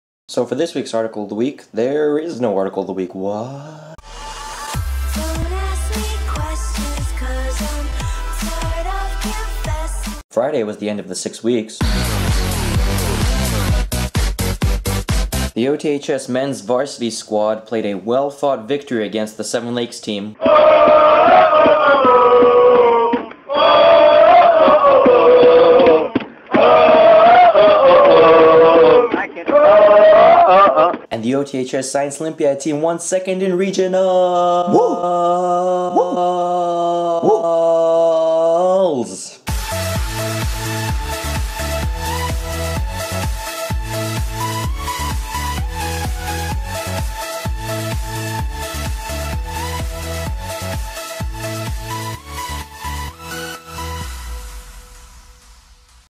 AoW Vlog :: No Article? Whaaaaa
In this week's thrilling episode, our hero has been left void of an article to talk about and must make his own path in this world.

Don't miss this short-but-sweet AoW Vlog!

Jokes aside, this was an amazing weekend for OTHS overall, both athletically and academically. And I think that is more than worthy to let celebrate.

At the same time, this just means we have even more work ahead of us, but I am confident the Falcons have got what it takes to succeed!